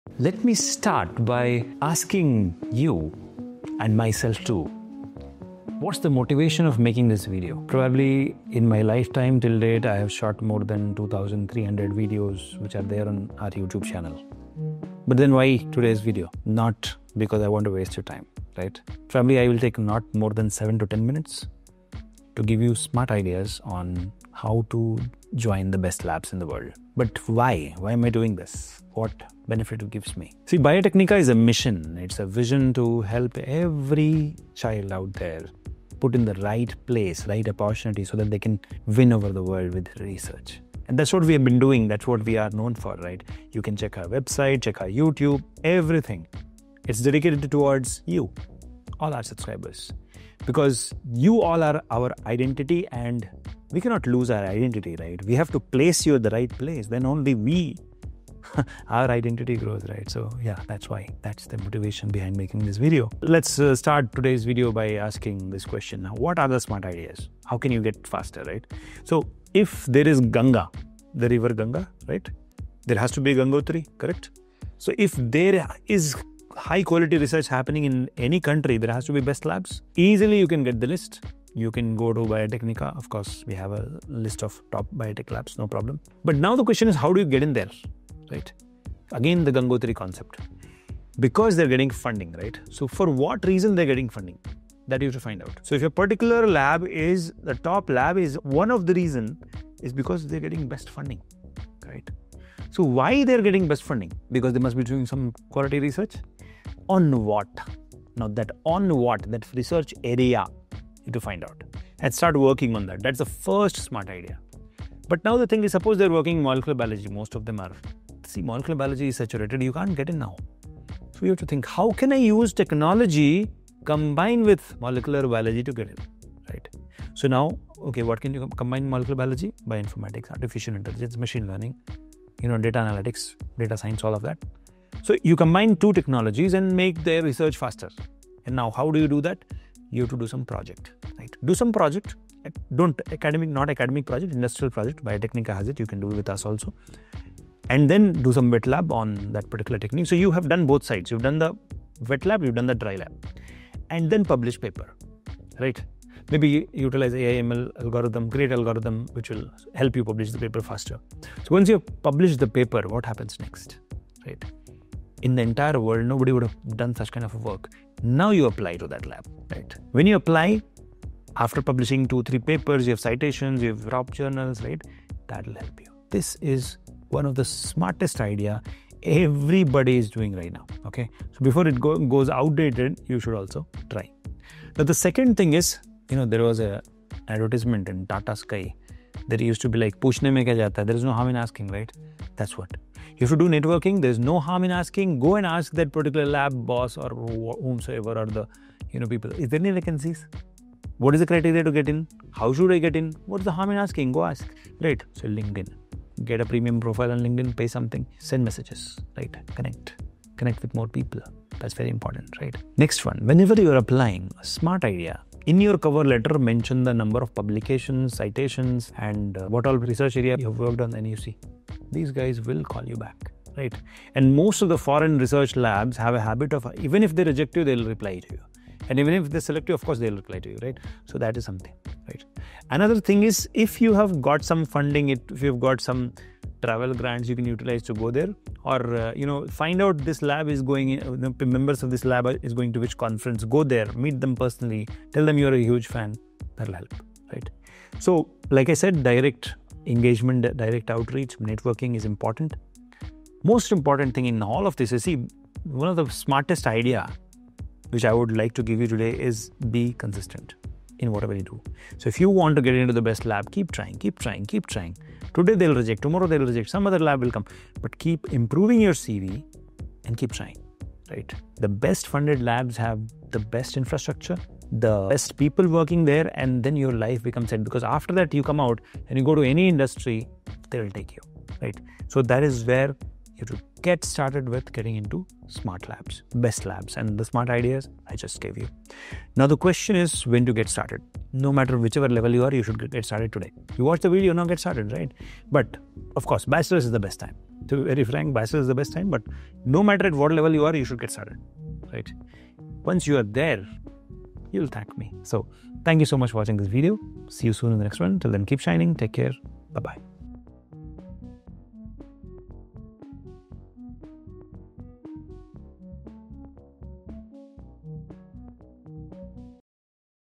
Smart Ideas to Get Into Top Biotech Labs - Even as a Fresher
Are you aspiring to work in a leading biotech lab but unsure where to start? In this video, we break down smart, strategic steps to help you land a position in a top biotech research lab. Whether you're a student, researcher, or professional looking to transition into biotech, these insights will help you navigate the competitive landscape.

Students, fresh graduates, research scholars, and professionals seeking to enter high-impact biotech labs.

🔔 Stay updated with the latest job openings, workshops, and industry insights! 🚀

👉 Join Biotecnika and turn your passion into a profession!